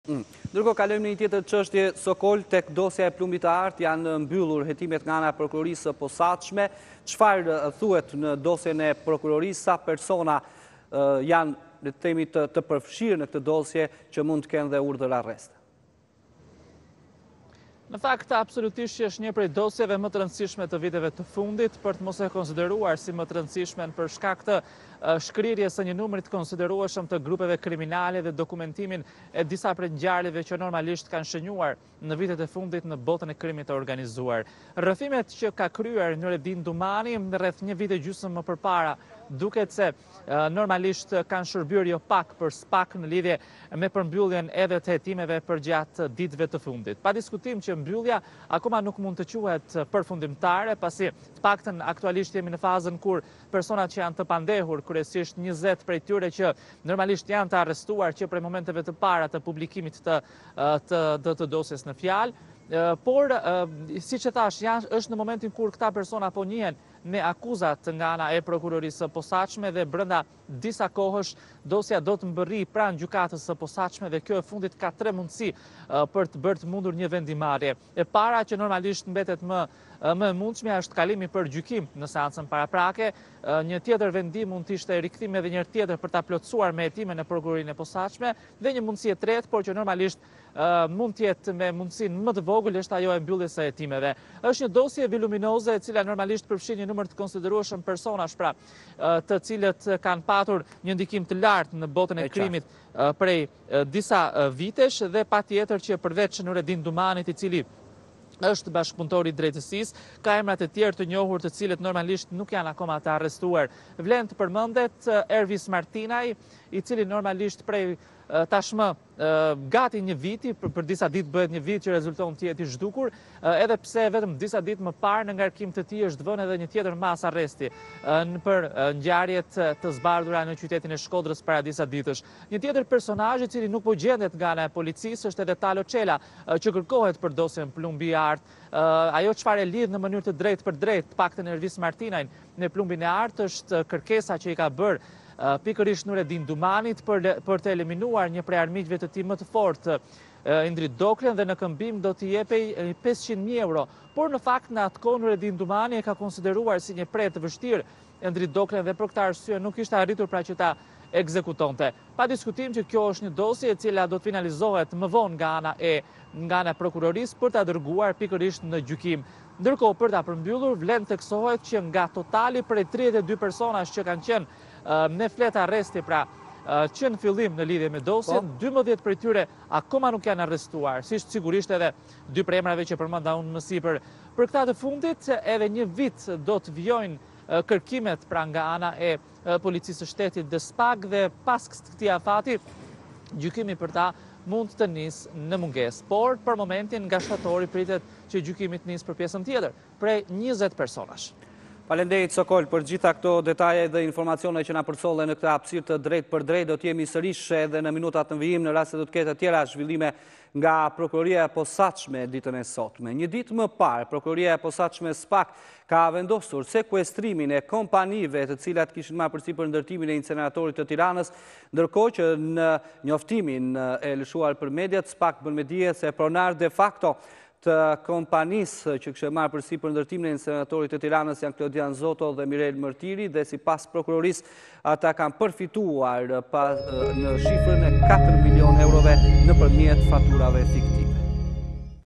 "Plumbi i Artë'/ SPAK përfundon hetimet,do i komunikohet akuza Nuredin Dumanit dhe 19 të tjerë
SPAK përfundon hetimet për dosjen e të penduarit Nuredin Dumani, i arrestuar në kuadër të operacionit "Plumbi i Artë", ku për një vit e gjysmë më parë u goditën disa grupe kriminale, dhe u lëshuan mbi 32 urdhër arreste.